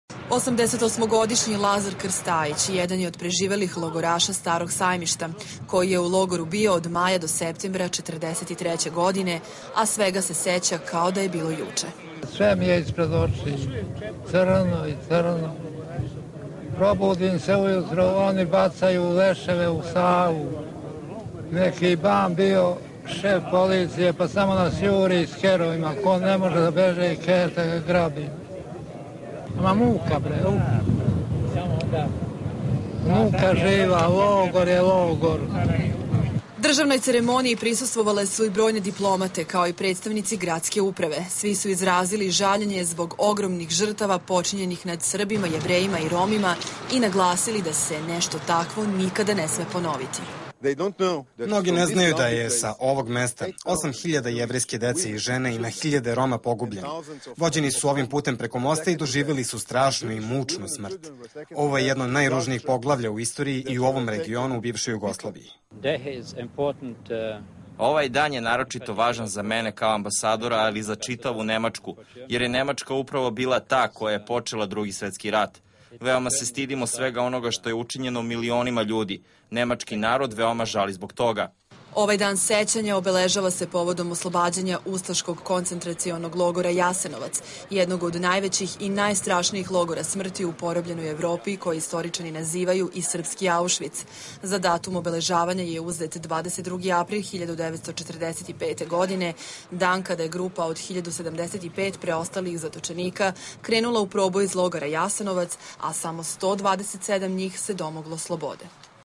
Obeležen Dan holokausta
U Srbiji je obeležen Dan sećanja na žrtve Holokausta i genocida u Drugom
svetskom ratu polaganjem venaca kod spomenika u kompleksu nekadašnjeg
nacističkog logora "Staro sajmište".